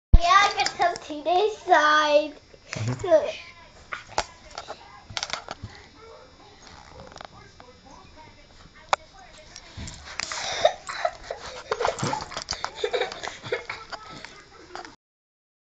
Megan, west highland white terrier
About 12 weeks old in this clip, very playful ... my son is always giggling when she's around.